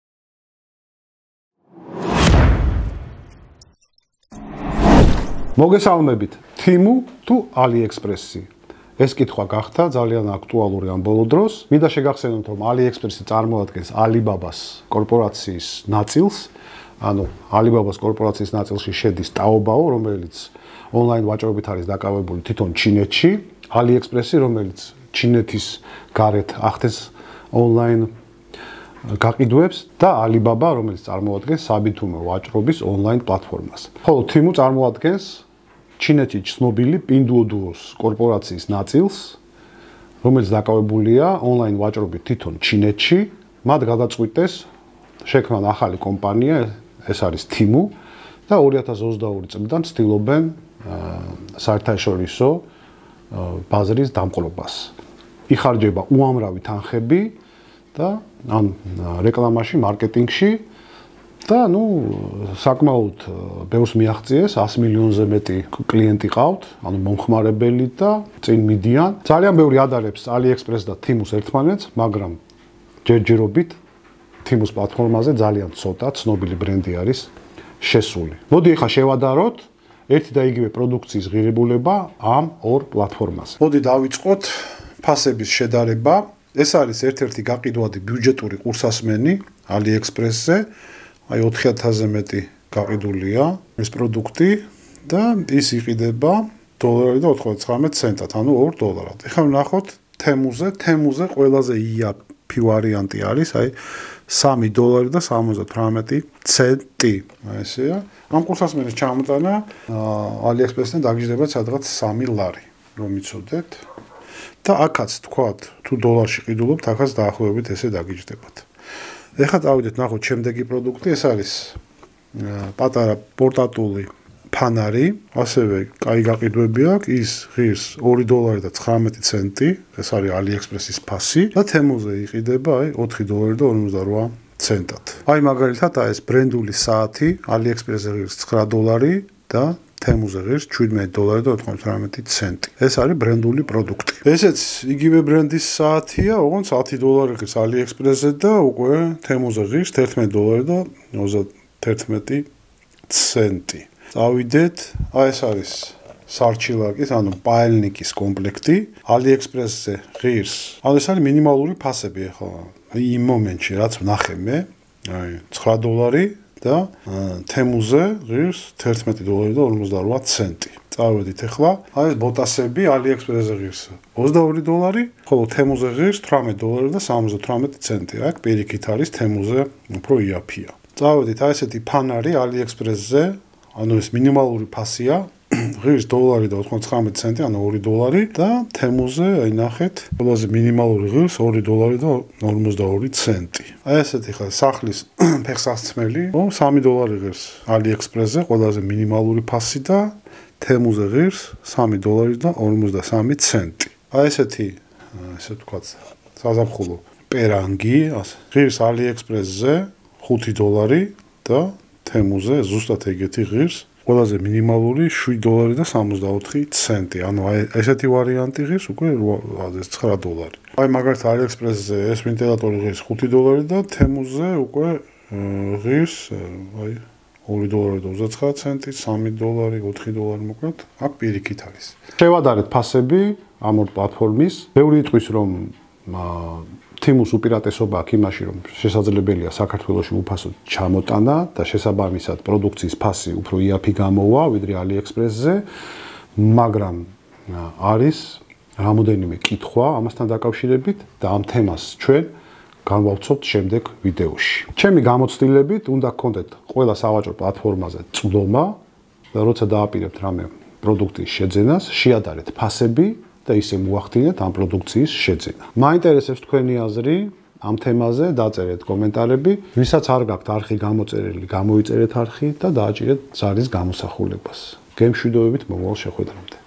TEMU თუ AliExpress-ი| ნაწილი 1| ფასების შედარება
TEMU თუ AliExpress-ი, ფასების შედარება , იაფი ამანათი, ფასდაკლება, ბიუჯეტური ყურსასმენი, სპორტული ფეხსაცმელი, სახლის ფეხსამელი, LIGE-ს საათი,